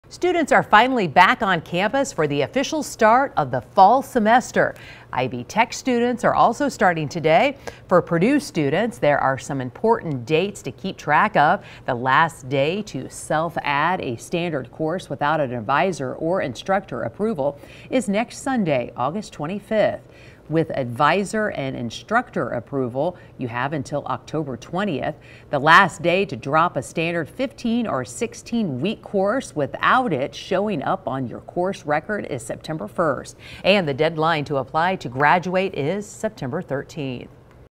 Purdue Classes Begin Today
Students are finally back on campus for the official start of the fall semester